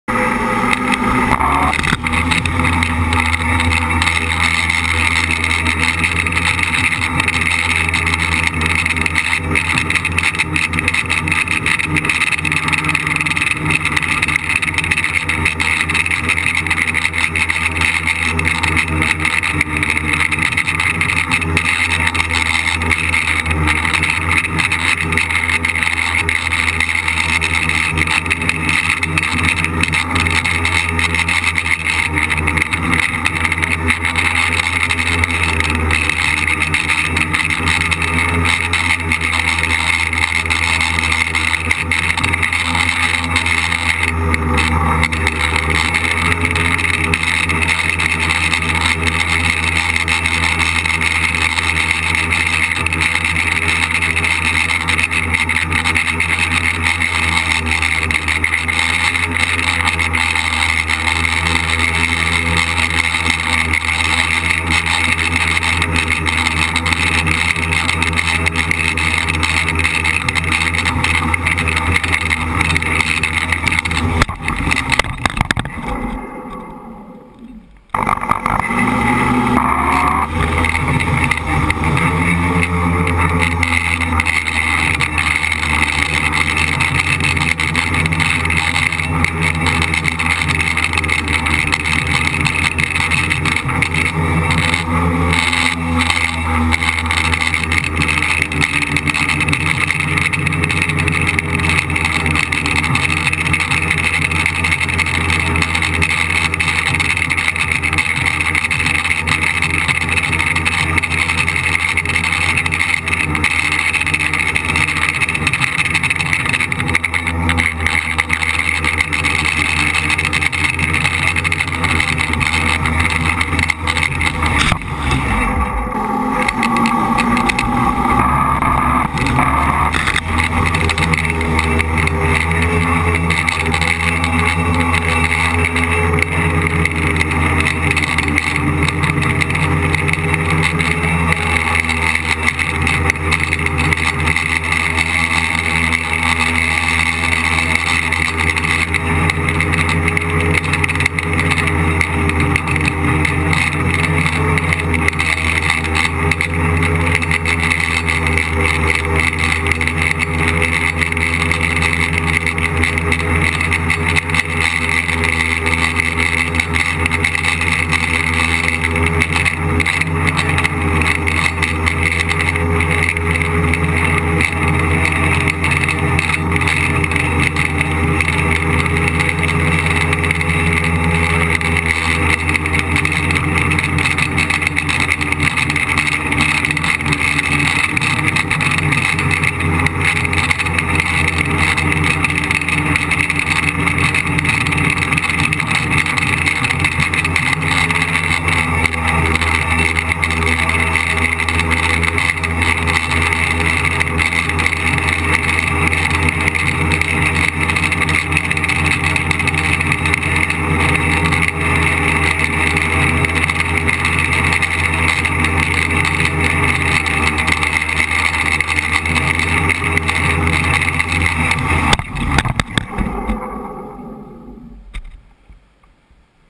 Crazy In Park
Tricopter (KK) FPV in hill part in Oslo, Norway